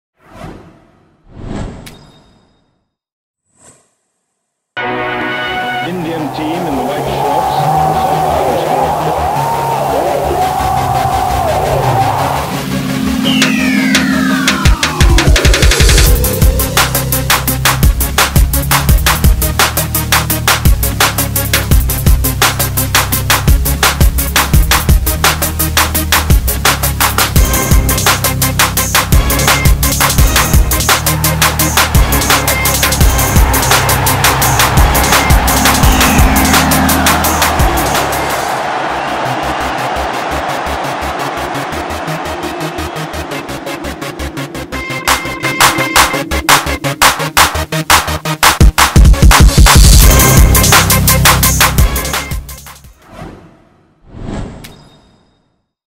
India's amazing hockey record - time to start winning again
India's hockey team has won eight golds, one silver and two bronze medals at the Olympics... but nothing since 1980. It's time to end the drought at Rio 2016.